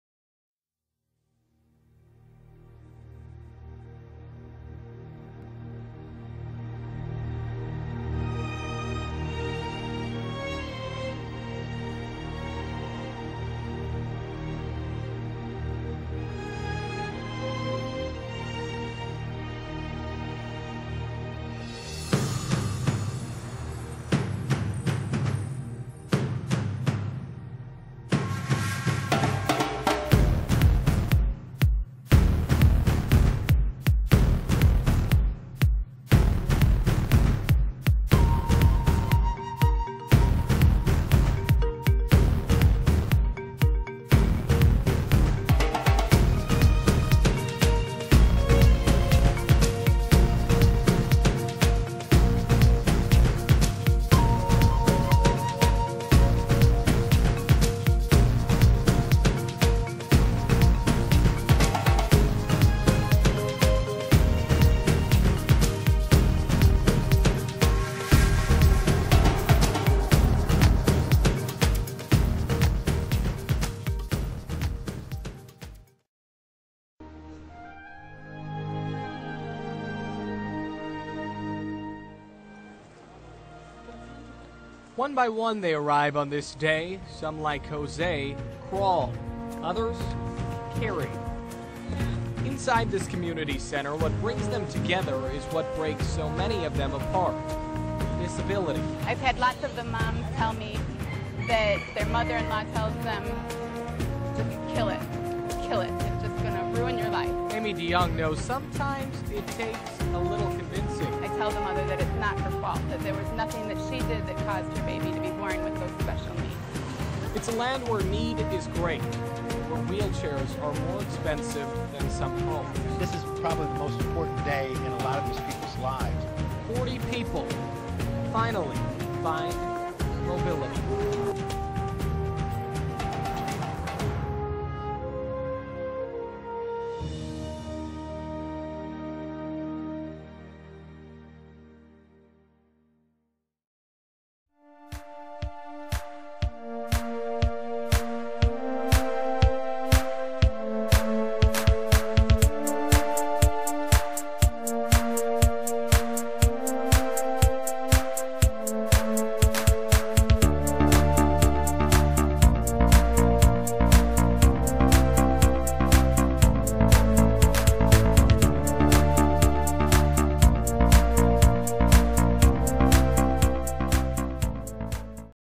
What happens to children born with disabilities in Third World nations?
About Jessica's Story ...
This is Jessica's Story.
Jessica is one of six children, her mother is a widow.
Jessica has Cerebral Palsy.

Jessica was taken to Hope Haven Guatemala (Refugio de Esperanza de Guatemala),
where Mark Richard's organization provides wheelchairs to disabled people who otherwise must crawl all their lives.
Ref [Will open in a new browser window.]

Dick Rutgers was at Hope Haven Guatemala when Jessica appeared.
The Lord touched Dick's heart.
This story shows the result.

Dick saw a young girl, only six years old and weighing only sixteen (16) pounds.
He had to do something, sometimes we simply cannot walk away.
Dick shared Jessica's Story with pictures on his website, Ref [Will open in a new browser window.]

KOMU-TV in Columbia, Missouri produced a television special about Hope Haven Guatemala.
The video portion of this production is excerpted from that TV special.
I was struck by the images and video.
You will be too.

Jessica's Story is a bitter but sweet story ... sad and happy.
Jessica's eyes tell the story.
Look into her eyes, perhaps Our Father will touch you too.
Perhaps a little background information is acceptable? Watch this video which asks and answers the question, "What happens to children born with disabilities in much of the world?"

In Guatemala, as well as much of the undeveloped world,
 » the parents of disabled children cannot afford either wheelchairs or motorized vehicles,
 »  » hence the children must crawl in the dirt to attend school.

There are no paved roads.
 » During the rainy seasons (when school is in session),
 »  » the mothers must carry their children to school in their arms.

Often the mothers have other children, or must work to feed their family.
 » These severely disabled children receive no education,
 »  » the children are not able to find employment, even when they grow up.
 »  »  » As a result of their disabilities, these severely disabled children must spend their entire lives as shut-ins at the homes of their desperately indigent parents.

Because there is absolutely no hope for these children to have a future,
 » and because they present an enormous life-long burden on their parents,
 »  » usually the mothers of disabled newborn babies are URGED by their families to kill their children immediately after their birth.

The significance of this documentary video: Amy DeYoung tells us from her own experience what happens to children born with disabilities in much of the world, their parents are pressured to euthanize the child for the sake of their family, and for its own well-being.
Reference Timeline: 00:01:37 [one minute thirty seven seconds] into this video.
Speaking in the video: Amy DeYoung explains what happens in developing nations (third world countries) to children who are born with disabilities. Amy states, [quote]"I have had lots of the moms tell me that their mother-in-law tells them 'Just kill it. Kill it! It's just going to ruin your life.' "[end quote]

Please consider the work of His hands. Help support our charity, the Work of God, at »
Please note: We must be very careful how we use HIS work, because Jessica and many other people are now in HIS hands, under HIS watchful and caring eye.